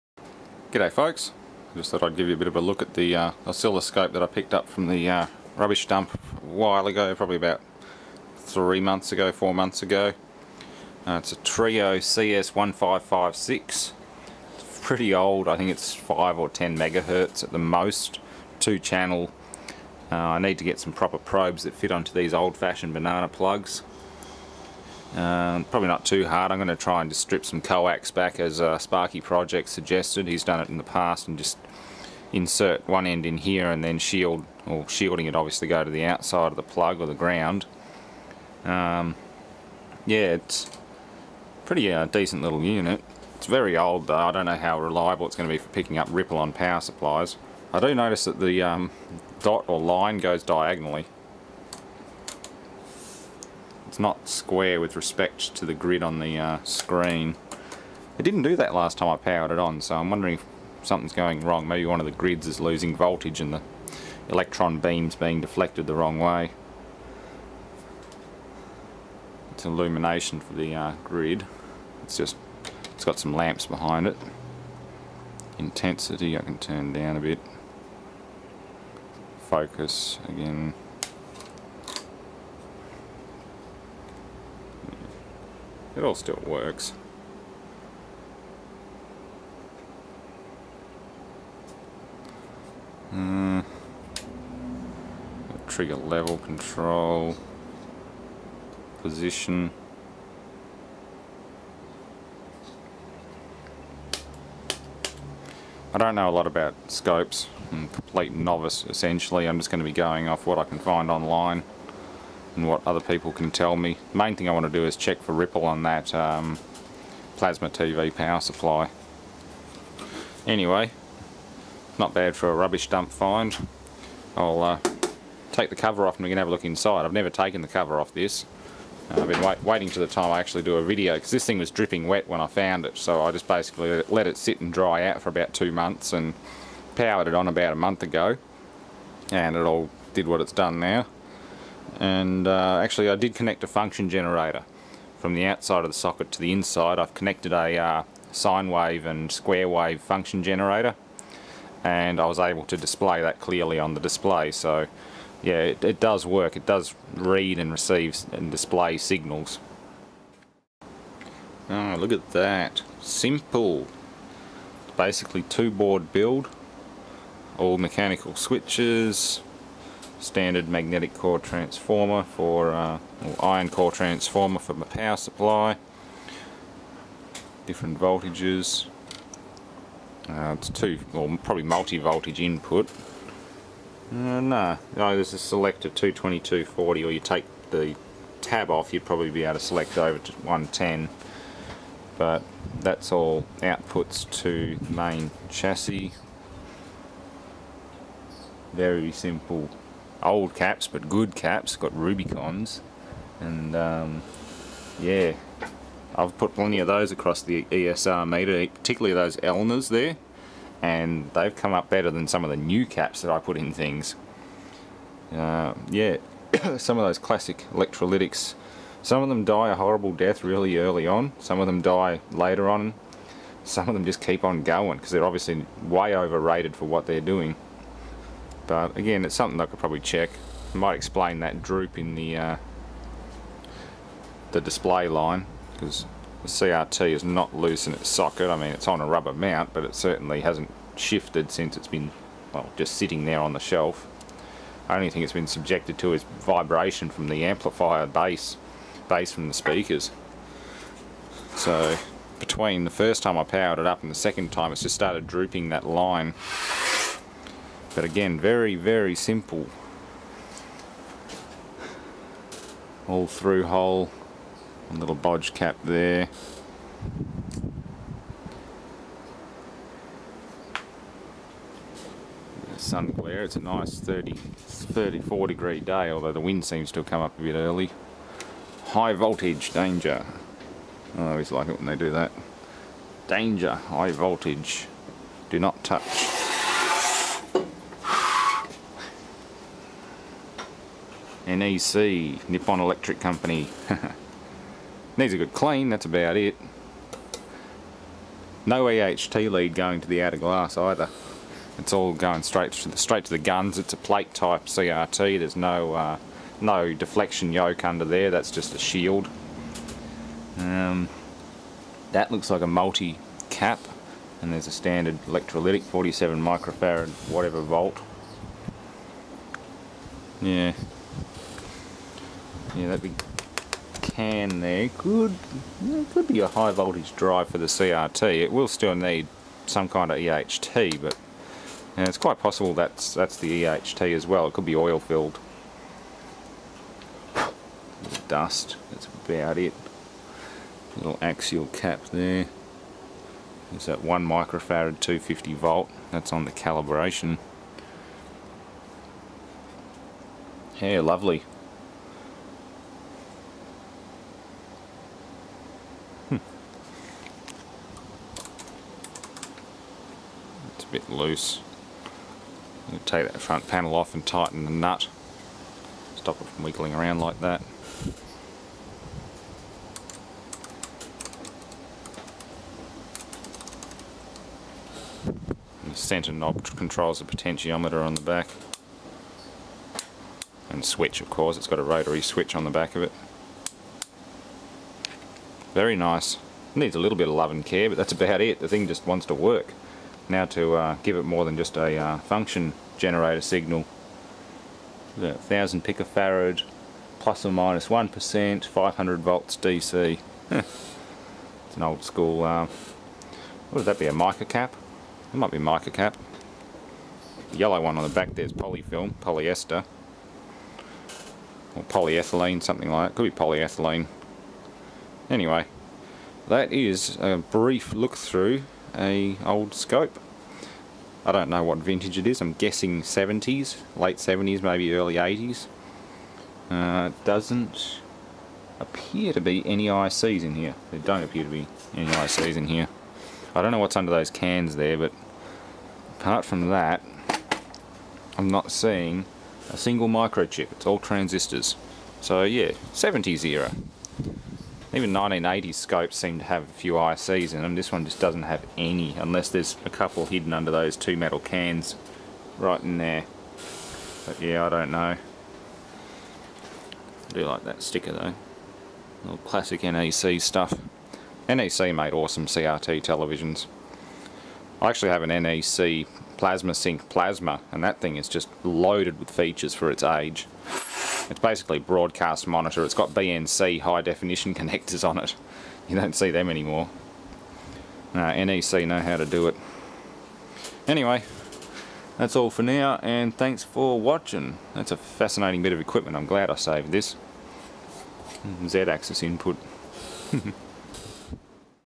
Vintage Trio Oscilloscope CS 1556
Scrap find from a local dump. not too bad at all!.